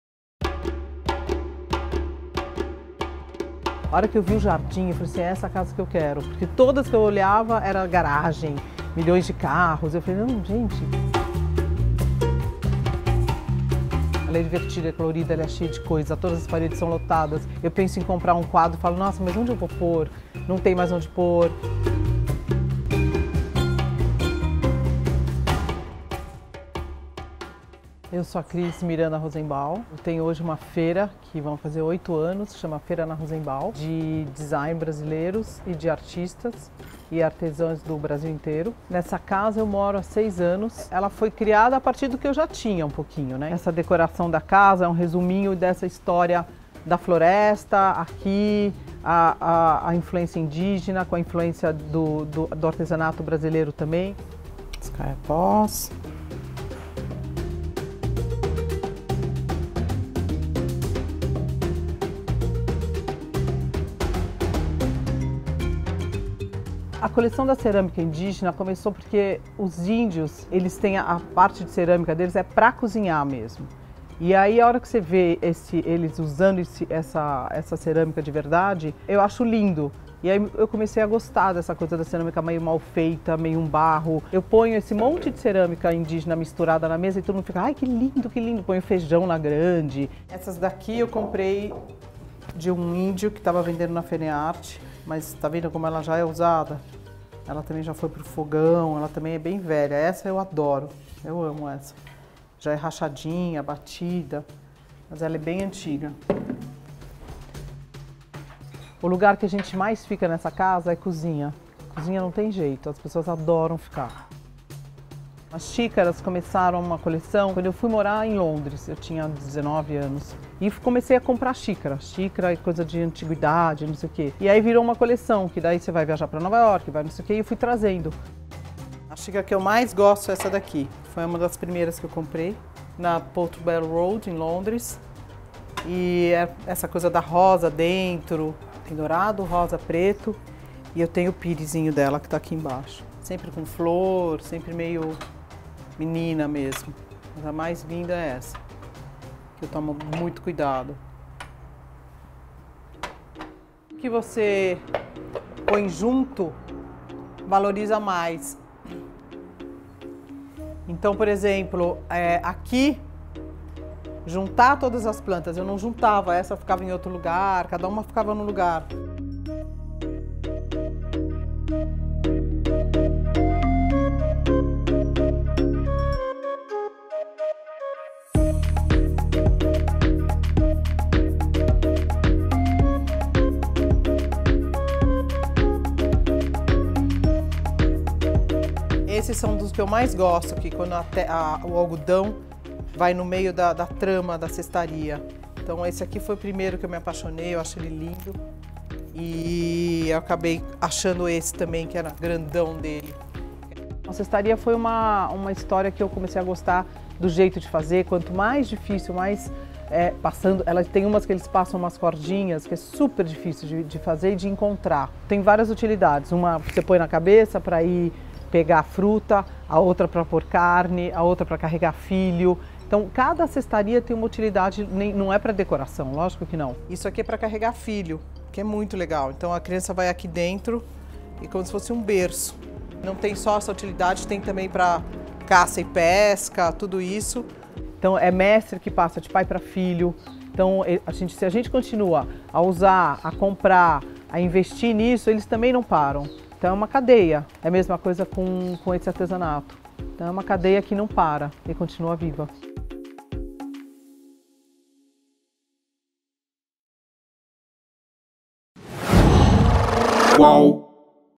CASA COM DECORAÇÃO BEM BRASILEIRA: CERÂMICA, CESTOS E QUADRINHOS | CASA DE ESTIMAÇÃO EP. 1
Conheça nossa primeira "Casa de Estimação", da designer Cris Rosenbaum. A casa é colorida, divertida, com as paredes cheias de objeto. Cris se inspirou na decoração indígena e no artesanato brasileiro para deixar tudo do gosto dela. Vem ver!